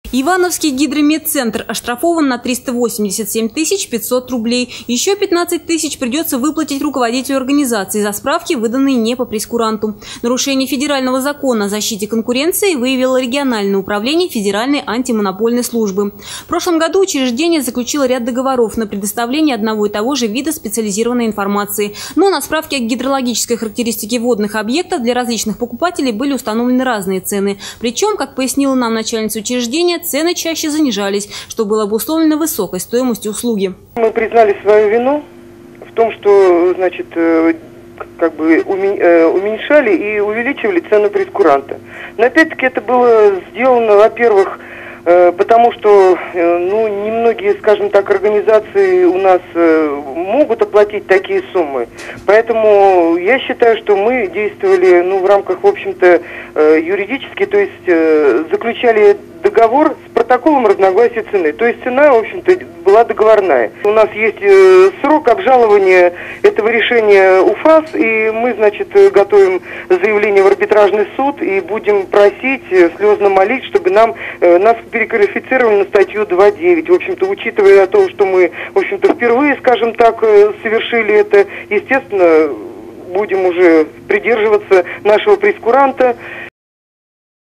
Гидрометцентр заставят заплатить
Ивановский Гидрометцентр  оштрафован на 387 тысяч 500 рублей. Ещё 15 тысяч придется выплатить руководителю организации за справки, выданные не по прейскуранту